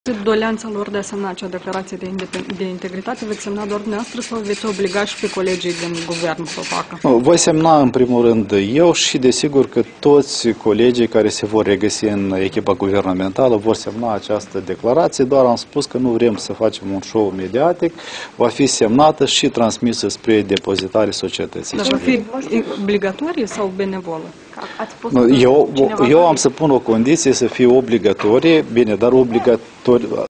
Declarațiile prim-ministrului desemnat, Pavel Filip, după consultările cu reprez...
Titlul evenimentului: Declarațiile prim-ministrului desemnat, Pavel Filip, după consultările cu reprezentanții patronatelor și ai societății civile